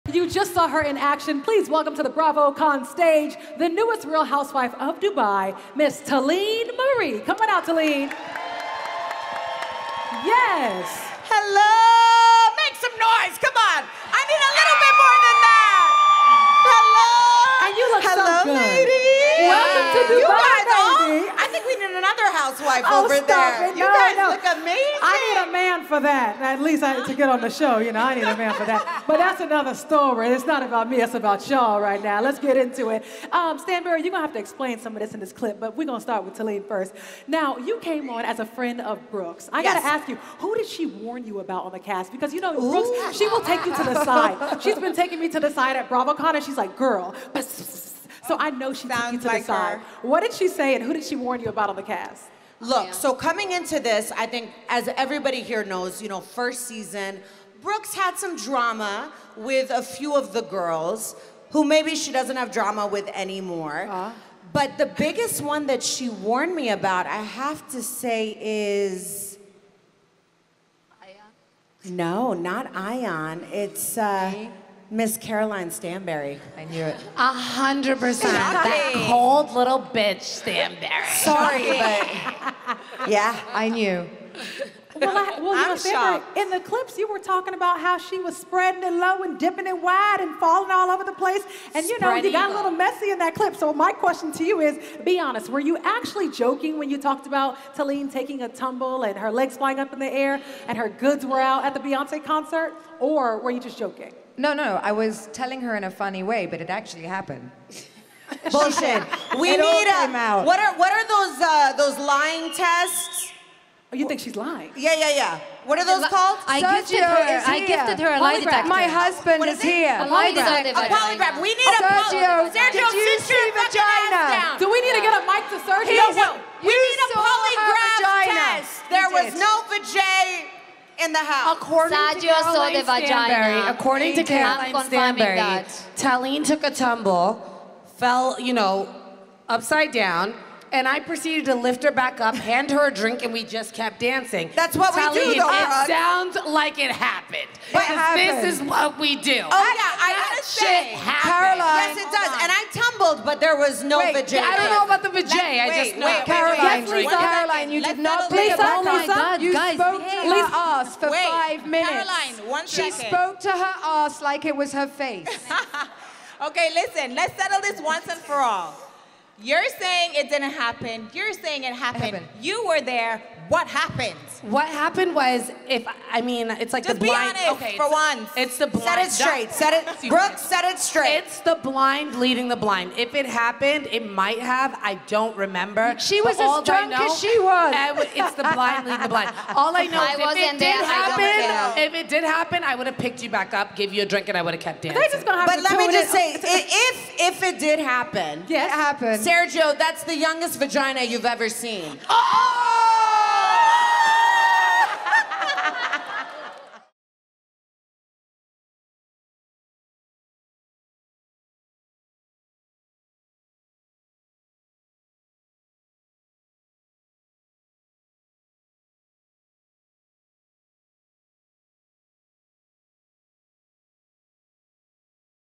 New Dubai Housewife Taleen Marie Sets the Beyonce Concert Drama Straight | BravoCon 2023
Taleen Marie takes the stage as the newest addition to the Dubai housewives' circle, but sparks fly as Taleen and Caroline Stanbury debate what really happened at the Beyonce concert.

As the latest city to join the Housewives universe, The Real Housewives of Dubai reunite to talk about adjusting to life as Housewives, where their friendships currently stand and tease the upcoming season and new addition to the group.
Moderator: Francesca Amiker